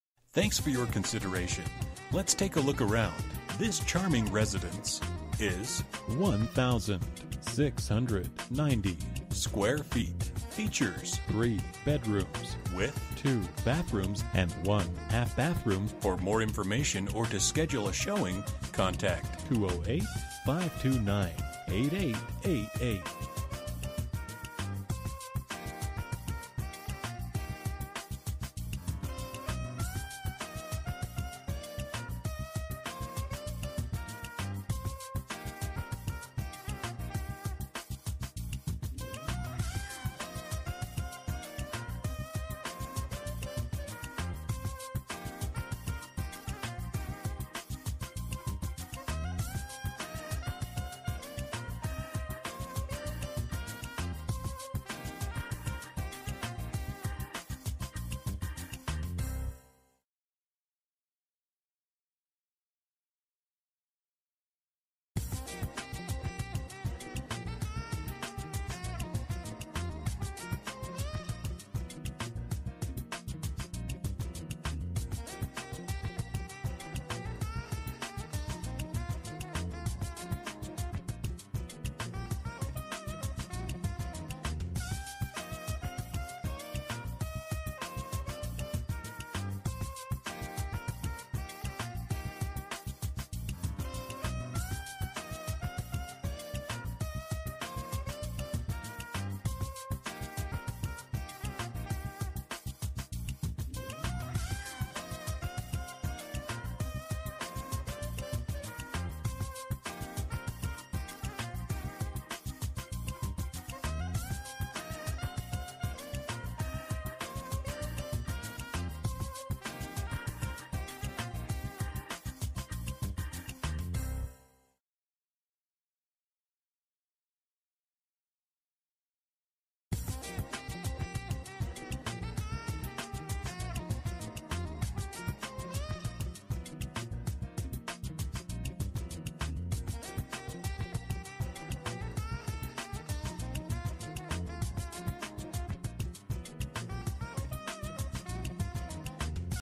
Homes for sale - 207 N Cottonwood Lane, BLACKFOOT, ID 83221
Welcome to 207 Cottonwood Lane! This beautiful property is the quintessence of Idaho living! Nearly ten irrigated pasture acres sit adjacent to the home and barn located on an additional five acres, making it ideal for horses and cattle. The home has been recently updated with new flooring, a bright white kitchen, beautiful alder vanities, tiled floors and showers. There are multiple outbuildings, including a huge 36x48 horse barn. Barn includes a large tack room and separate office, 6 horse stalls, 36x16 hay shed, 36x14 covered front storage area, and has a lifetime warranty. There are multiple corrals, a loading chute, cattle working facilities, 2 automatic stock tanks and a horse walker. The house, barn and irrigation are all metered separately. Property has two separate entrances, including a private drive right off of Hwy 91. The secluded yard features hundreds of mature trees, a large garden with greenhouse, and a private pond with waterfall leading up to the entrance of the home. 18 water shares and 1 division right are included with the property.

Bedrooms: 3
Bathrooms: 2 full and 1 half
Square Feet: 1690
Price: $300,000